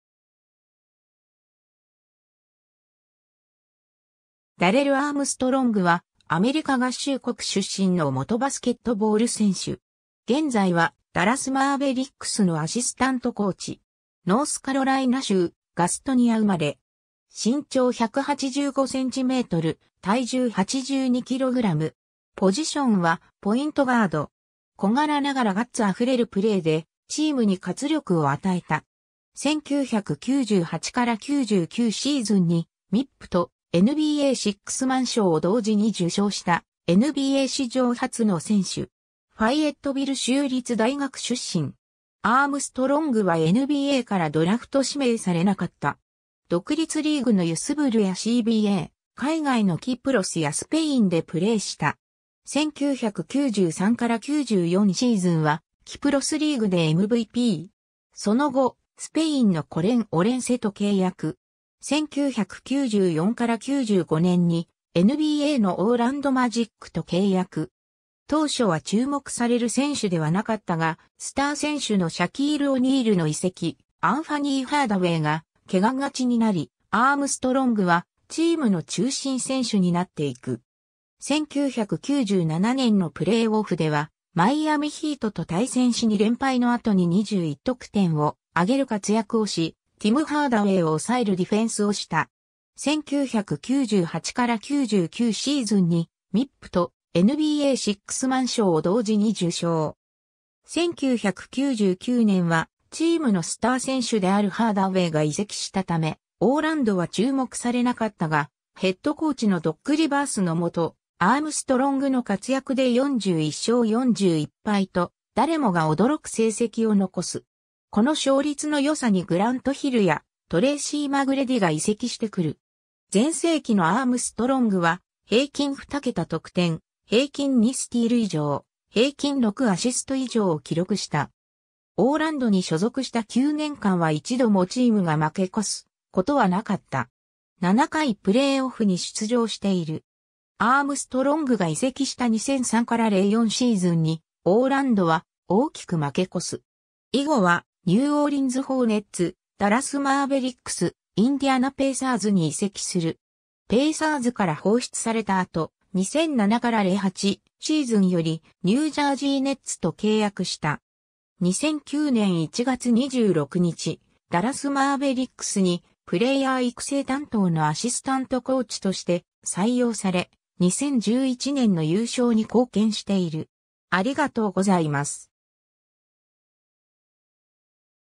ダレル・アームストロング
ニューオーリンズ・ホーネッツの選手
ニュージャージー・ネッツの選手
ダレル・アームストロング（Darrell Eugene Armstrong, 1968年6月22日-）はアメリカ合衆国出身の元バスケットボール選手。
現在はダラス・マーベリックスのアシスタントコーチ。
ノースカロライナ州ガストニア生まれ。
小柄ながらガッツ溢れるプレーでチームに活力を与えた。
1998-99シーズンにMIPとNBAシックスマン賞を同時に受賞したNBA史上初の選手。
 ファイエットビル州立大学出身。
アームストロングはNBAからドラフト指名されなかった。
独立リーグのUSBLやCBA、海外のキプロスやスペインでプレーした。
1993-94シーズンはキプロスリーグでMVP。
その後、スペインのコレン・オレンセと契約。
 1994-95年にNBAのオーランド・マジックと契約。
当初は注目される選手ではなかったが、スター選手のシャキール・オニールの移籍、アンファニー・ハーダウェイが怪我がちになり、アームストロングはチームの中心選手になっていく。
1997年のプレイオフでは、マイアミ・ヒートと対戦し2連敗の後に21得点をあげる活躍をし、ティム・ハーダウェイを抑えるディフェンスをした。
1998-99シーズンにMIPとNBAシックスマン賞を同時に受賞。
1999年は、チームのスター選手であるハーダウェイが移籍したためオーランドは注目されなかったが、ヘッドコーチのドック・リバースの元、アームストロングの活躍で41勝41敗と誰もが驚く成績を残す。
この勝率のよさにグラント・ヒルやトレイシー・マグレディが移籍してくる。
全盛期（1998～2000）のアームストロングは、平均二桁得点、平均2スティール以上、平均6アシスト以上を記録した。
オーランドに所属した9年間は1度もチームが負け越すことはなかった。
7回プレイオフに出場している。
アームストロングが移籍した2003-04シーズンに、オーランドは大きく負け越す。
 以後は、ニューオーリンズ・ホーネッツ（2003-04年）、ダラス・マーベリックス（2004-06年）、インディアナ・ペイサーズ（2006-07年）に移籍する。
ペイサーズから放出された後、2007-08シーズンよりニュージャージー・ネッツと契約した。
 2009年1月26日、ダラス・マーベリックスにプレーヤー育成担当のアシスタントコーチとして採用され、2011年の優勝に貢献している。